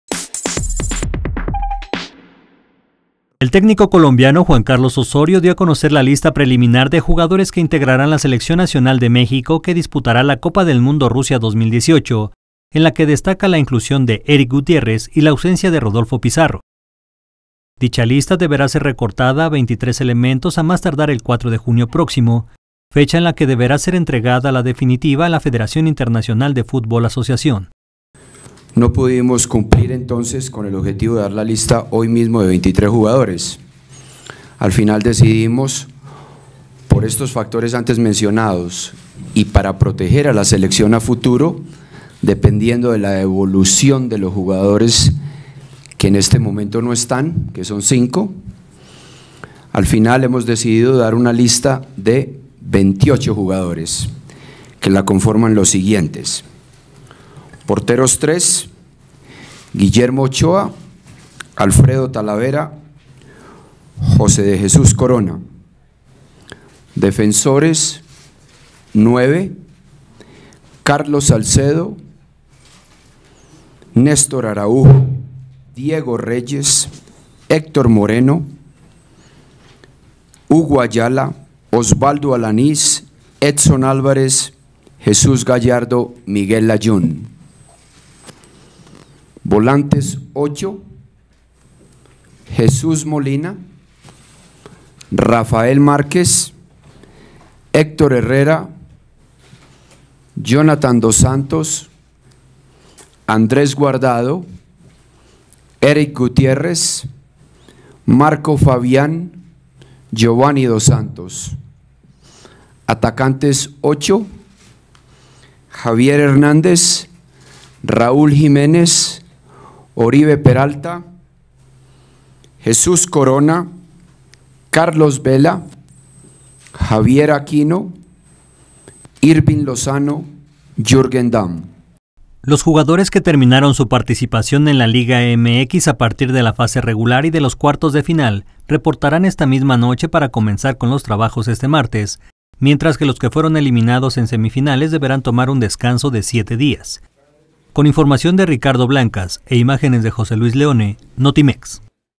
Osorio da a conocer lista preliminar del "Tri" para Rusia 2018
El técnico colombiano Juan Carlos Osorio dio a conocer la lista preliminar de jugadores que integraran la Selección Nacional de México.